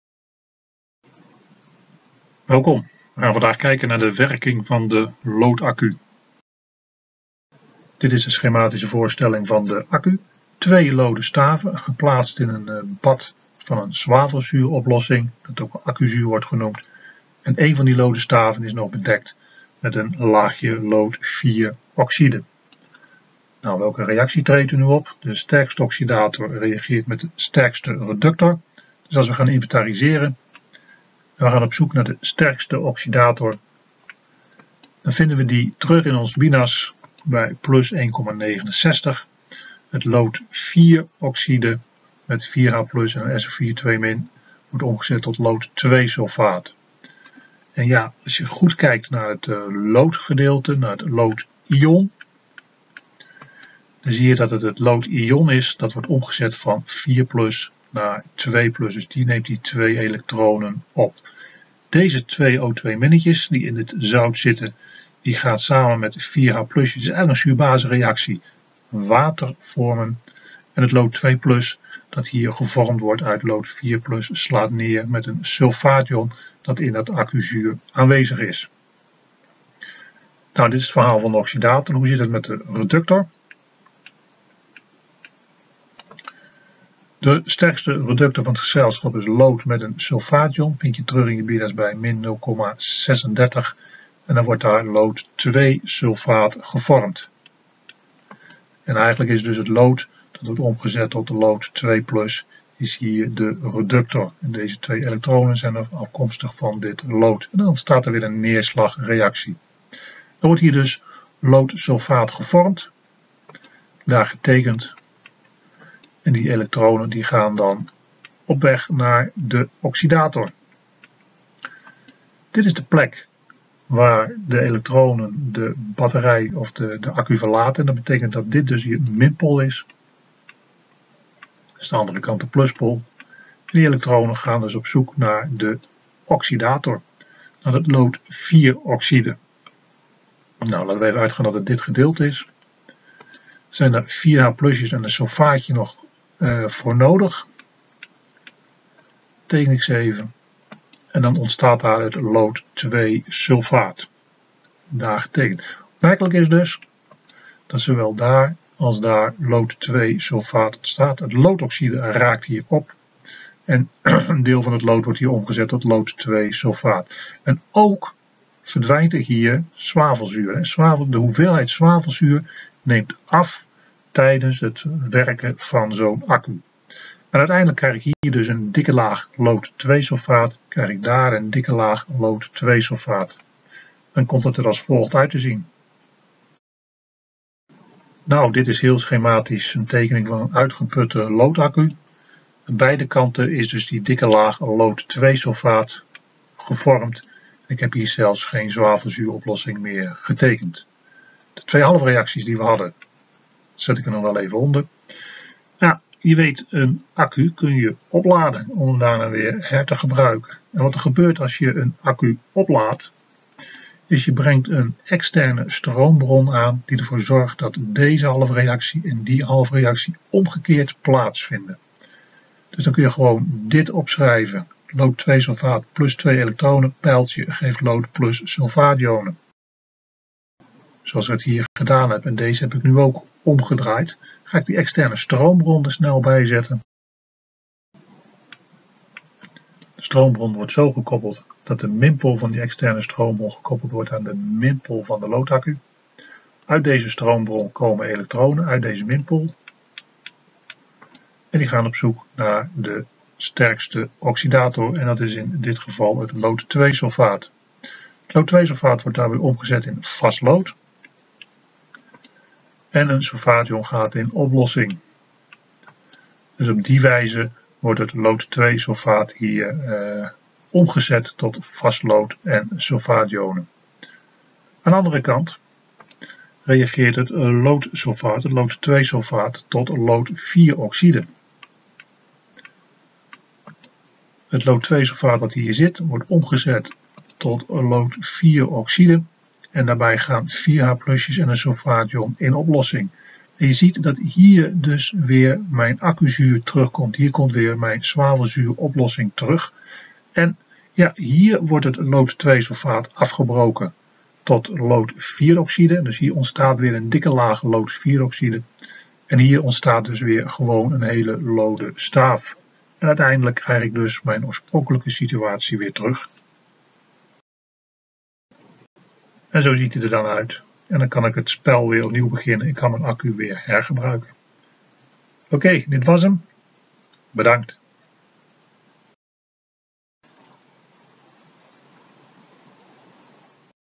Werking van de loodaccu
Uitleg van de werking van de loodaccu. Geschikt voor havo en vwo